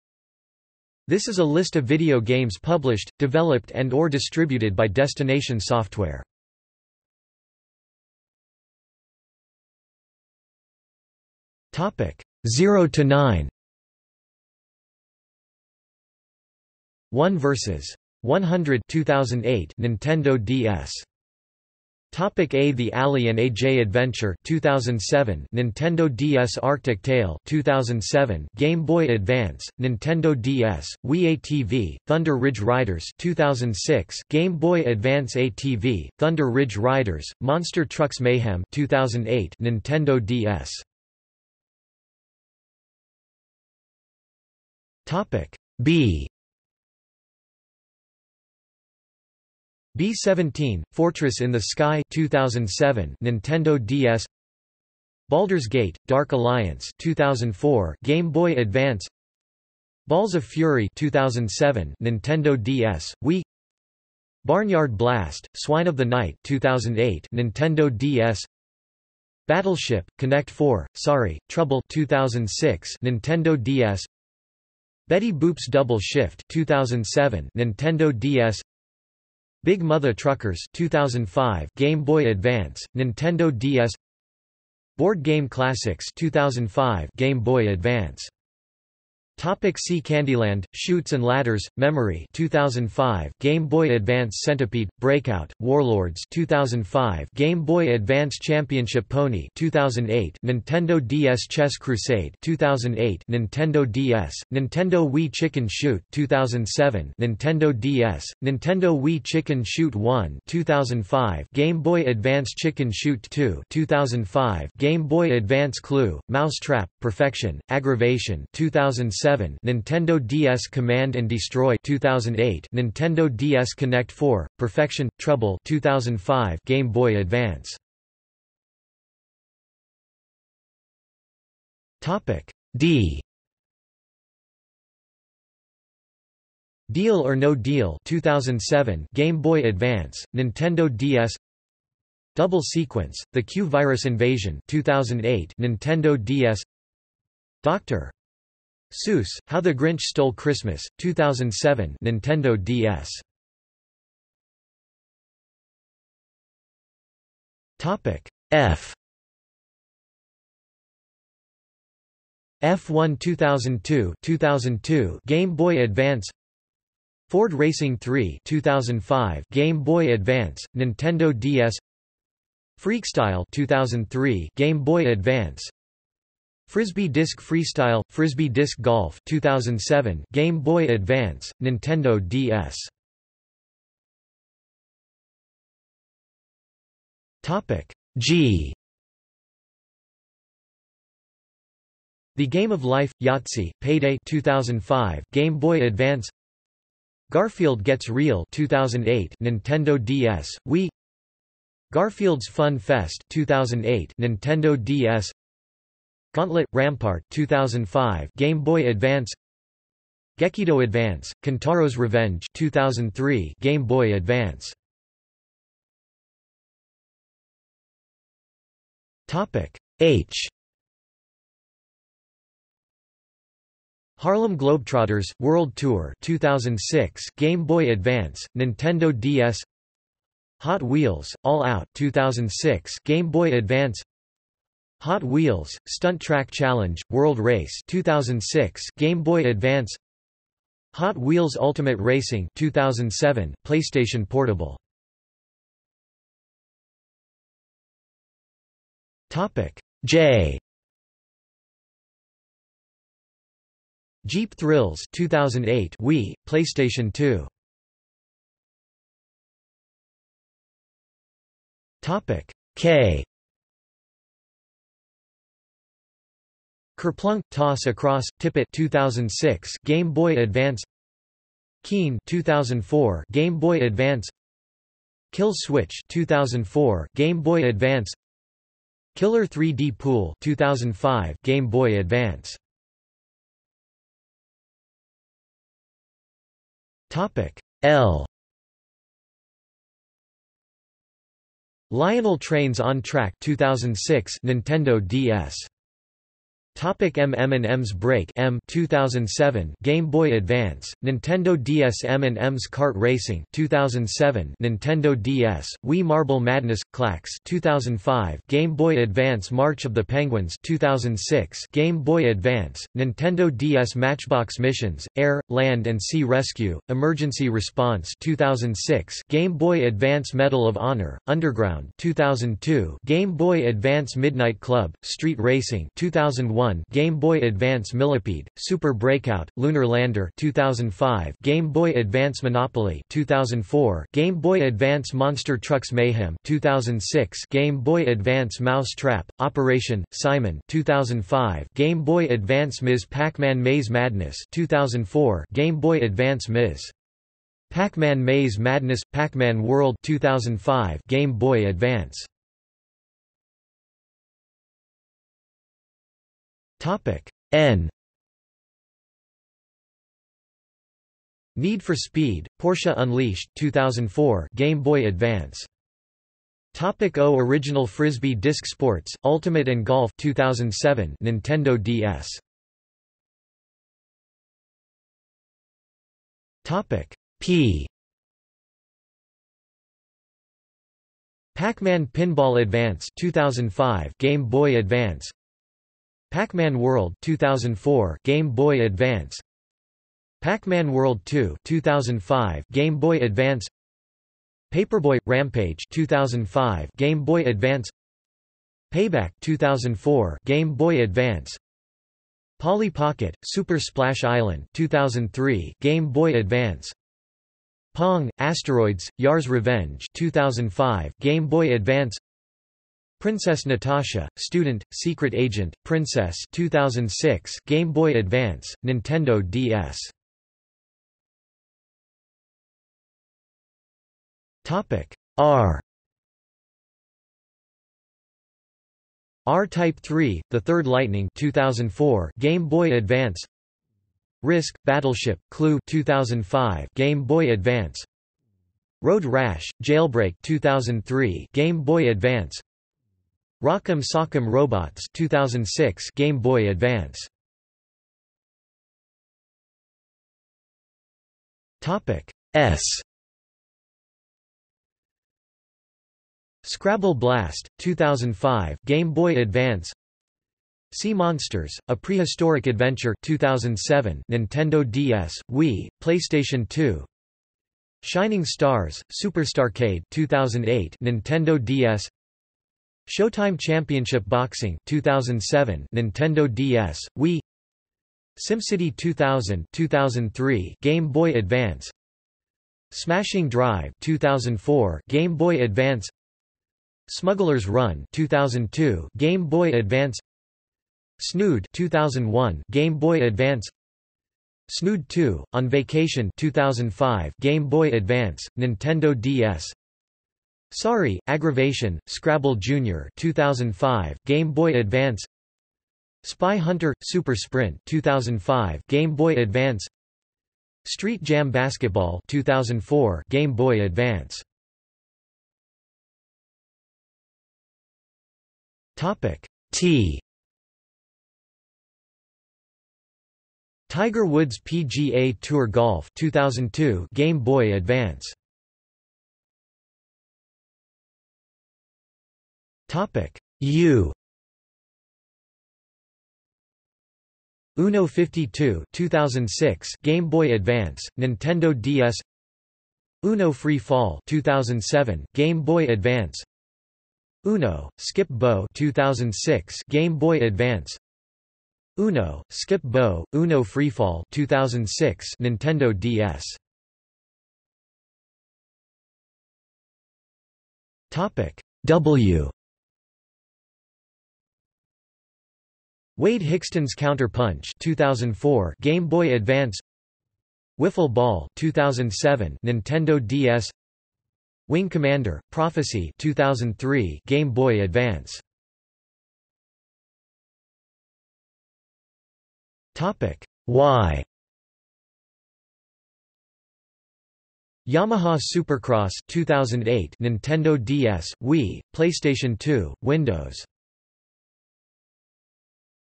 List of Destination Software games | Wikipedia audio article
00:00:08 0-9
00:00:22 A
00:00:41 B
00:01:29 C
00:02:08 D
00:02:34 F
00:03:03 G
00:03:35 H
00:04:05 J
00:04:18 K
00:04:43 L
00:04:56 M
00:05:53 N
00:06:06 O
00:06:13 P
00:07:01 R
00:07:28 S
00:08:35 T
00:08:49 U
00:09:17 W
00:09:39 Y

- Socrates

SUMMARY
This is a list of video games published, developed and/or distributed by Destination Software.